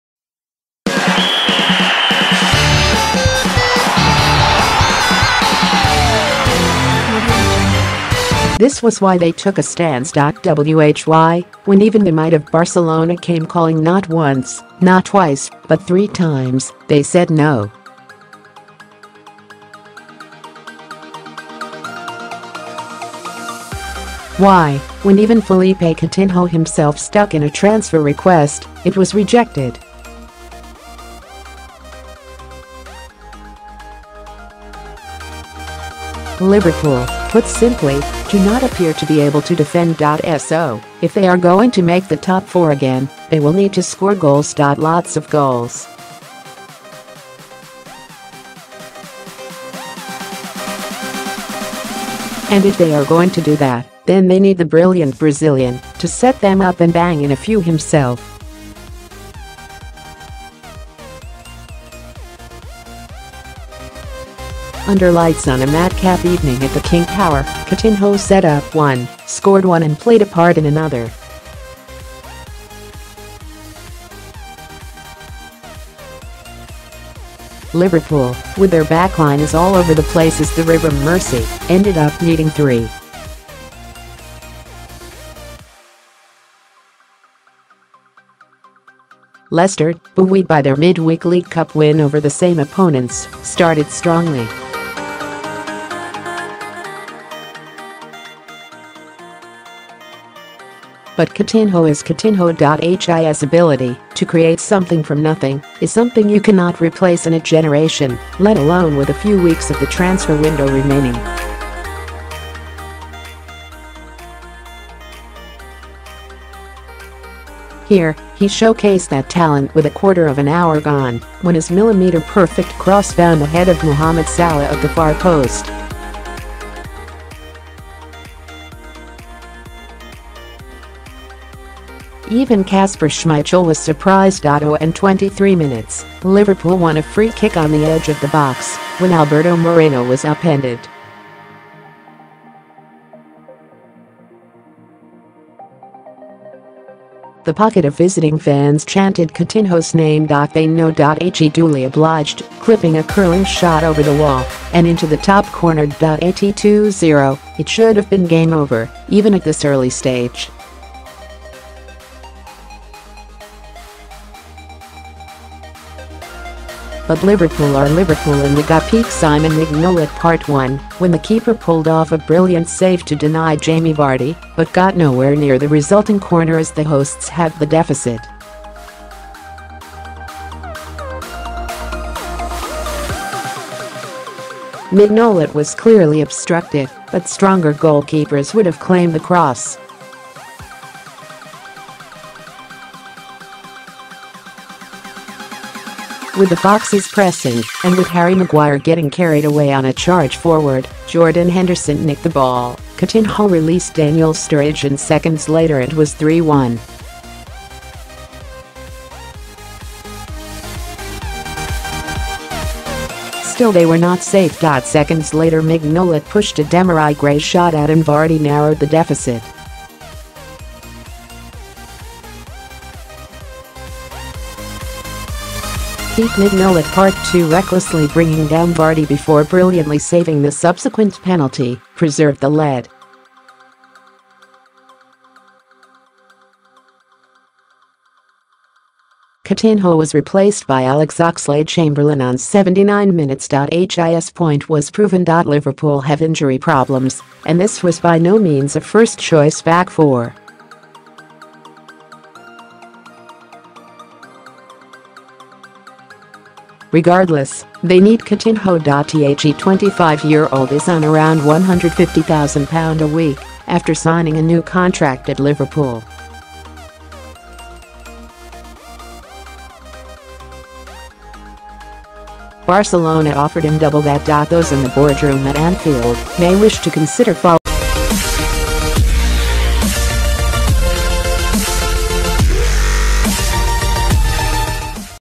Liverpool show why they refused to let Coutinho leave
This was why they took a stance.Why, when even the might of Barcelona came calling not once, not twice, but three times, they said no.Why, when even Philippe Coutinho himself stuck in a transfer request, it was rejected.Liverpool, put simply, do not appear to be able to defend.So if they are going to make the top four again, they will need to score goals.Lots of goals.And if they are going to do that, then they need the brilliant Brazilian to set them up and bang in a few himself.Under lights on a madcap evening at the King Power, Coutinho set up one, scored one and played a part in another.Liverpool, with their backline as all over the place as the River Mersey, ended up needing three.Leicester, buoyed by their midweek League Cup win over the same opponents, started strongly.But Coutinho is Coutinho.His ability to create something from nothing is something you cannot replace in a generation, let alone with a few weeks of the transfer window remaining.Here, he showcased that talent with a quarter of an hour gone, when is millimetre-perfect cross found the head of Mohamed Salah at the far post.Even Kasper Schmeichel was surprised.On 23 minutes, Liverpool won a free-kick on the edge of the box when Alberto Moreno was upended.The pocket of visiting fans chanted Coutinho’s name.They know.He duly obliged, clipping a curling shot over the wall and into the top corner.At 2-0, it should have been game over, even at this early stage.But Liverpool are Liverpool and we got peak Simon Mignolet Part One when the keeper pulled off a brilliant save to deny Jamie Vardy but got nowhere near the resulting corner as the hosts halved the deficit.Mignolet was clearly obstructed, but stronger goalkeepers would have claimed the cross.With the Foxes pressing, and with Harry Maguire getting carried away on a charge forward, Jordan Henderson nicked the ball, Coutinho released Daniel Sturridge and seconds later it was 3-1.Still they were not safe.Seconds later Mignolet pushed a Demarai Gray shot out and Vardy narrowed the deficit.Peak Mignolet Part II, recklessly bringing down Vardy before brilliantly saving the subsequent penalty, preserved the lead.Coutinho was replaced by Alex Oxlade-Chamberlain on 79 minutes.His point was proven.Liverpool have injury problems and this was by no means a first choice back four.Regardless, they need Coutinho.The 25–year-old is on around £150,000-a-week after signing a new contract at Liverpool.Barcelona offered him double that.Those in the boardroom at Anfield may wish to consider following the Catalan club’s lead.